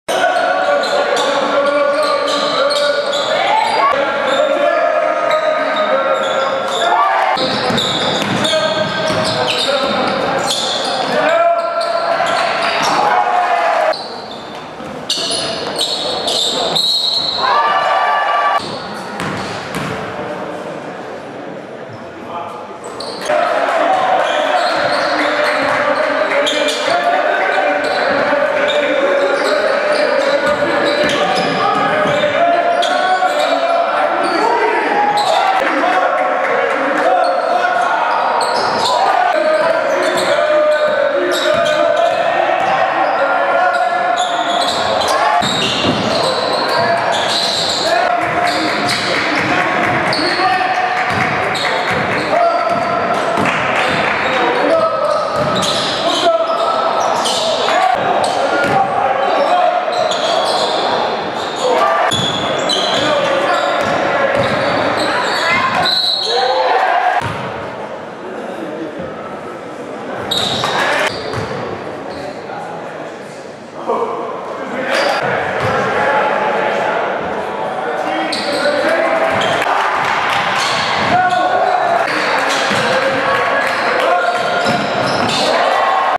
Grant Mullins with 37 vs. #1 Nationally ranked St. Mikes
Championship game of the Ontario All Catholic tournament.